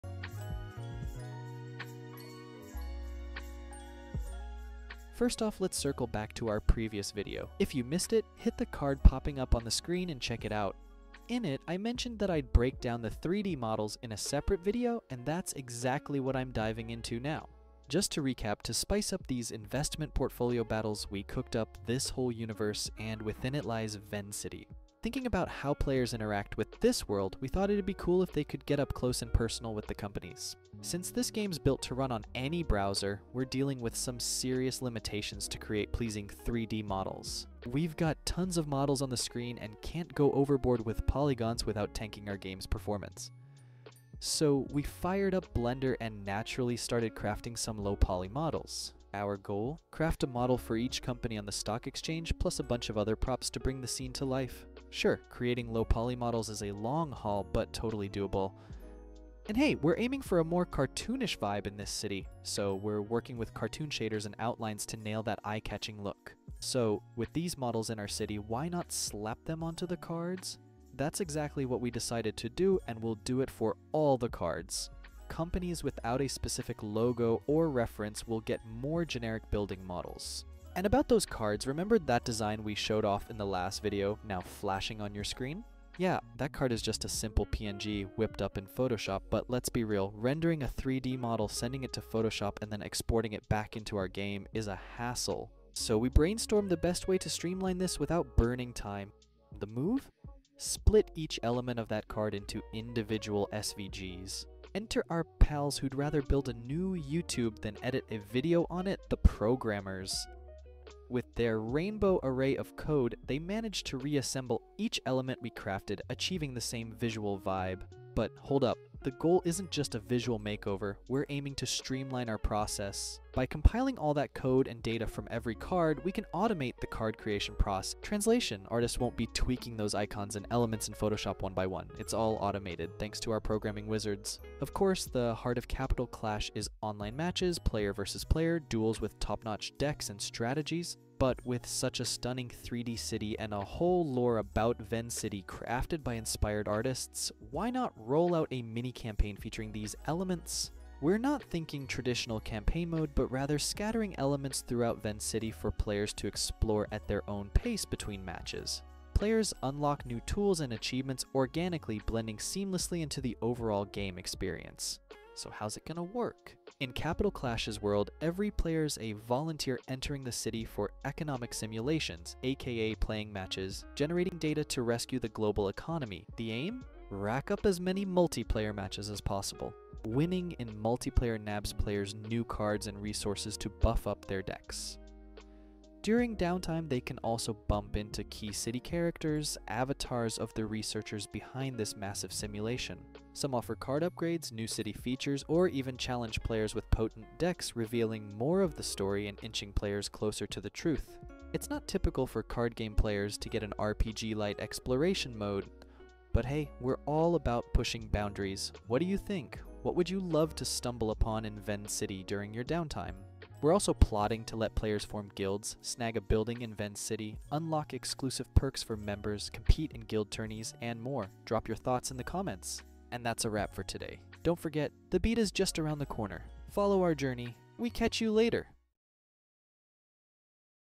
The Art of Low Poly 3D and Card Design for our Economy-Based Card Game | Devlog #2
Welcome back to our devlog series for 'Capital Clash,' where investment strategies clash with the dynamic universe of card gaming. In this second episode, we're plunging deeper into the digital streets of Venn city, showcasing our artistry and technical prowess in 3D modeling and card design.

In this log, we circle back to our first video to give you an exclusive breakdown of the 3D models that breathe life into Venn city. We discuss the challenges of creating visually appealing, low poly models for each company on the stock exchange, ensuring they not only enrich our universe but also maintain the game's performance. Our goal? To infuse a cartoonish charm into Venn city, using cartoon shaders and outlines for that vibrant, eye-catching appeal.

Then, we dive into the transformation of these models onto our game cards, detailing our innovative approach to streamlining the design process. We reveal how, with the magic touch of our programming team, we've automated the card creation process, saving precious time without compromising on the visual flair.

But there's more! We introduce a groundbreaking concept to blend our rich lore with gameplay. Imagine exploring Venn city's alleys and avenues between matches, uncovering secrets, meeting intriguing characters, and unlocking new tools and achievements. This isn't just a card game; it's an adventure that merges the thrill of player vs. player duels with an immersive story-driven campaign.

And for those who thrive in community and competition, we tease the upcoming feature of forming guilds, laying claim to a building in Venn city, and enjoying exclusive guild perks and tournaments.

So, what surprises would you love to encounter in Venn city? Your insights fuel our creativity. Don't miss out on the next stages of our journey – the BETA is just on the horizon.

We're not just building a game; we're crafting a world.